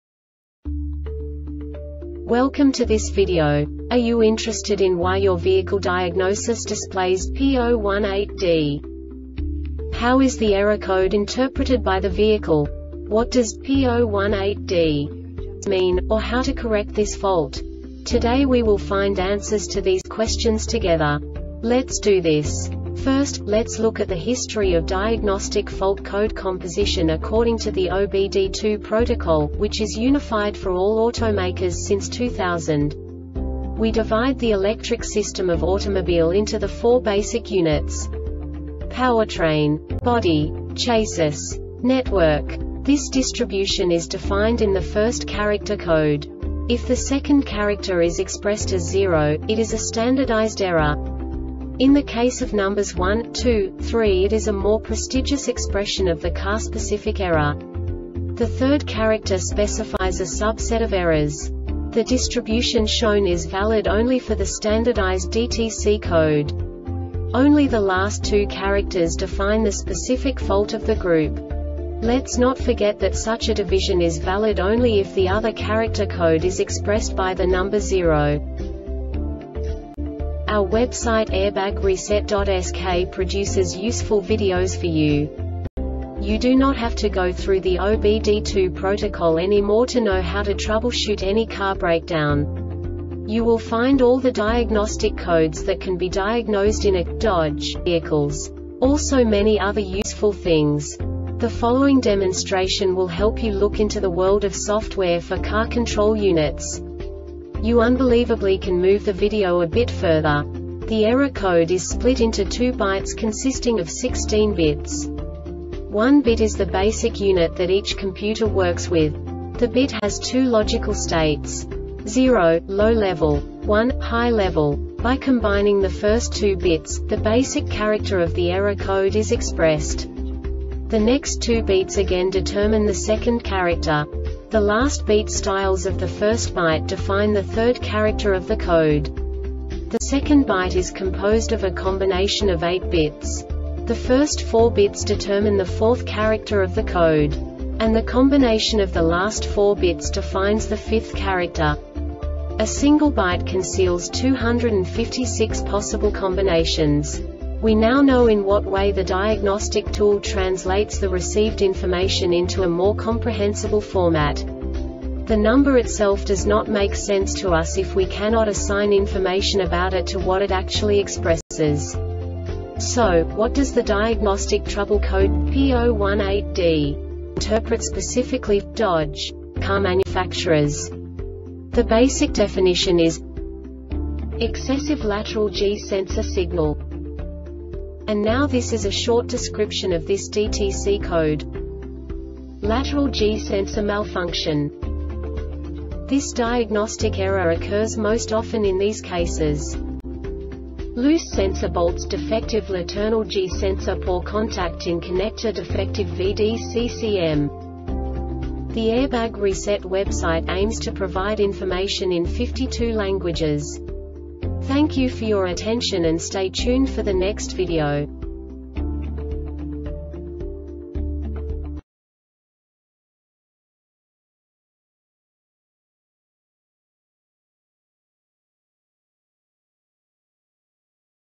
DTC Dodge P018D Short Explanation
Contents:
0:21 Basic DTC analysis according to OBD2 protocol standard.
1:48 Insight into programming
2:58 A diagnostically understandable description of the Diagnostic Trouble Code
3:05 Basic error definition

Make:
Dodge
Code:
P018D
Definition:
Excessive Lateral G Sensor Signal
Description:
Lateral G sensor malfunction
Cause:
Loose sensor boltsDefective laternal G sensorPoor contact in connectorDefective VDCCM